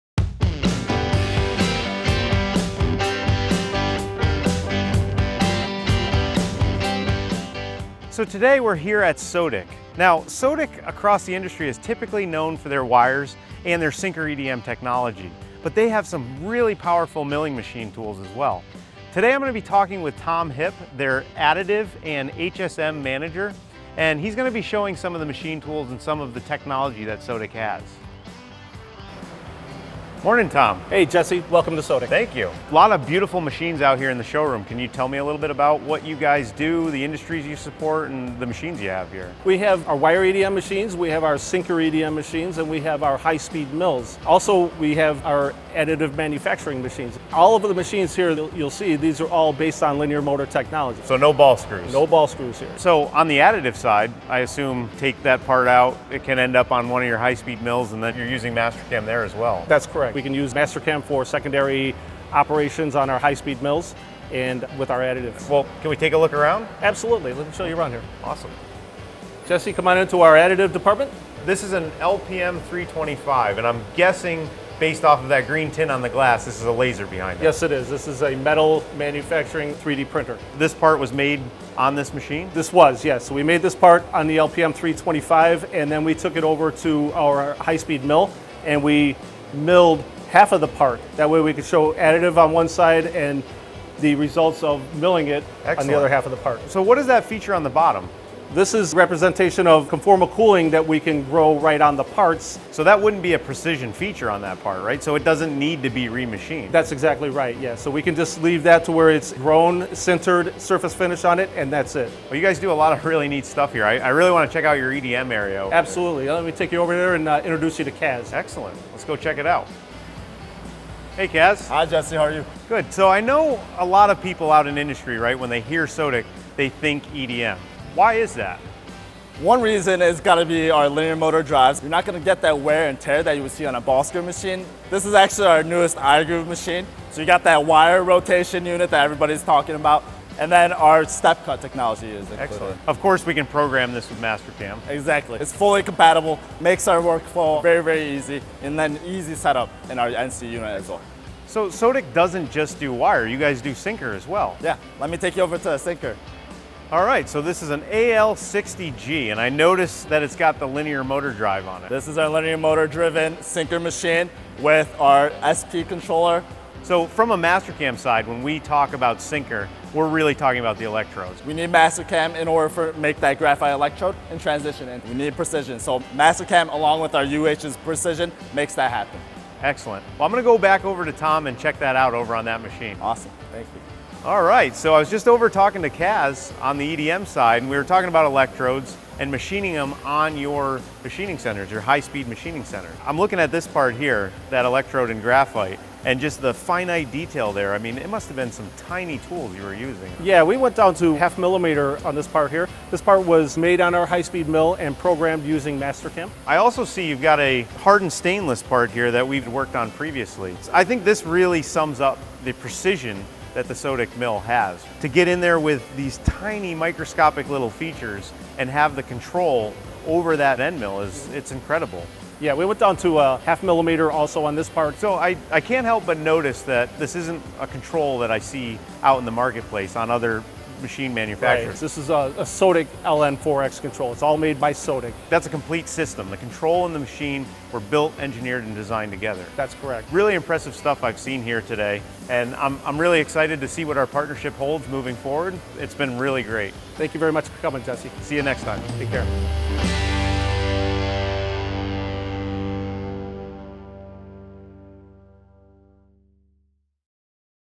Sodick and Mastercam | Meet our Manufacturing Partners
Jesse recently met with the @SodickUSA team to talk about how they're pairing their latest technology with Mastercam to manufacture precision parts.

Join the Mastercam community on social